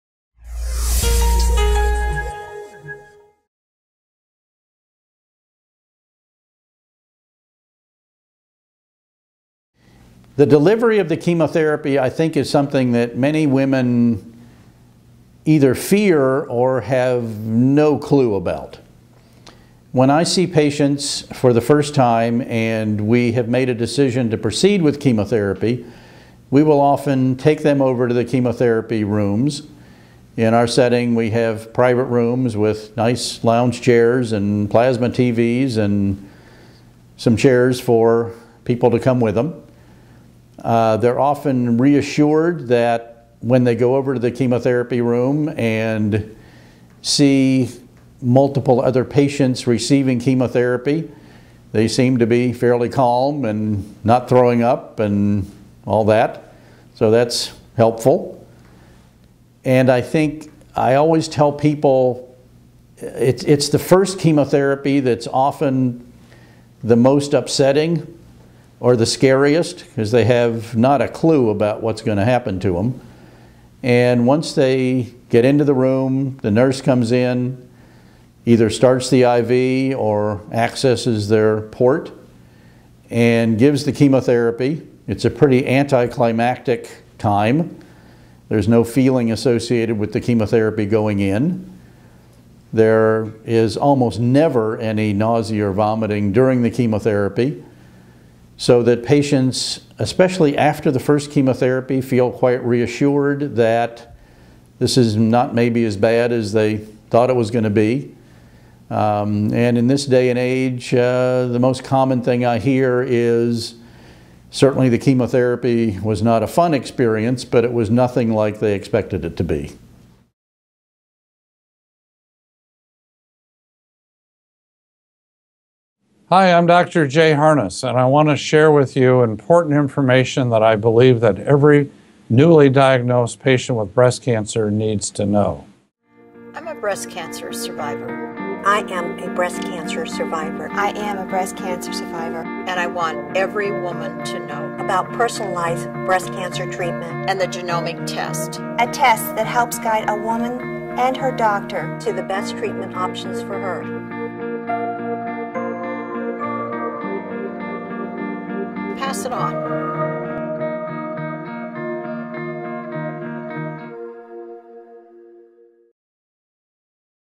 Chemotherapy Treatment For Breast Cancer
In this video, Dr. Margileth reassures patients that despite the fears of many women facing chemotherapy treatment, there is no feeling or pain associated when the chemotherapy is delivered through the IV.

The 15 Breast Cancer Questions To Ask Your Doctor.